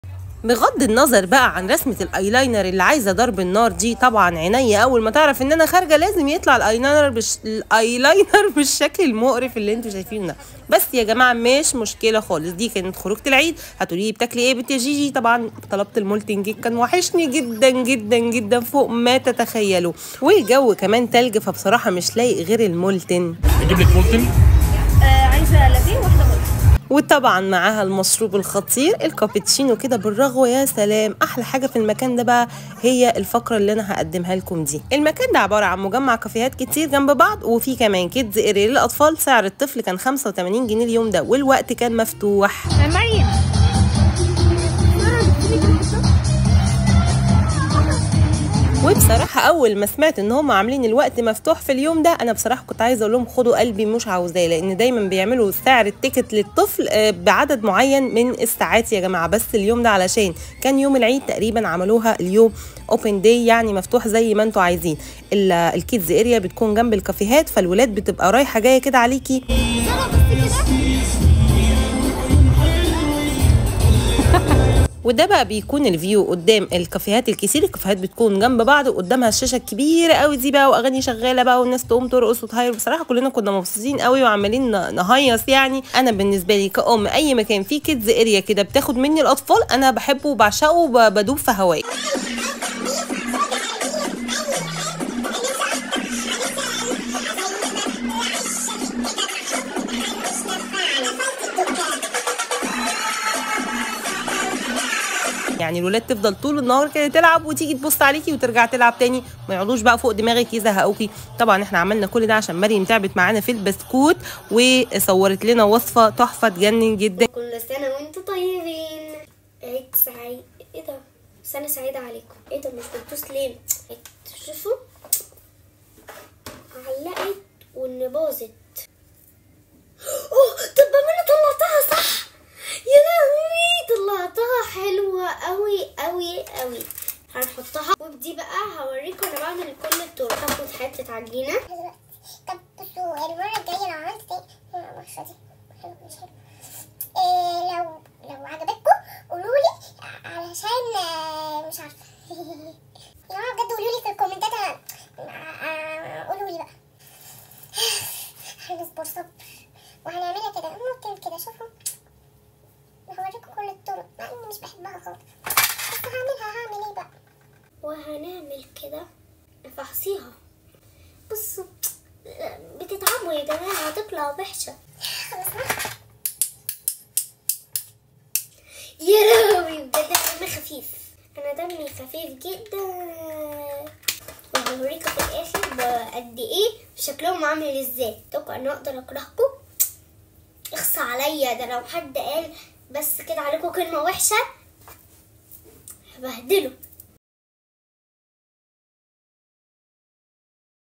رسمة الايلينر ال عاوزة ضرب النا*ر وقالتلي انا تعبت معاكي وشقيت لازم تخرجيني خروجة حلوة / فلوج العيد
ماعليك سوي الانتساب لهذه القناة لمشاهدة المزيد من الوصفات وللاستفادة من المزايا:-

كواي-kwai :-
‎ينشئ مدون الفيديو هذا مقاطع فيديو رائعة! تعالي وشاهد!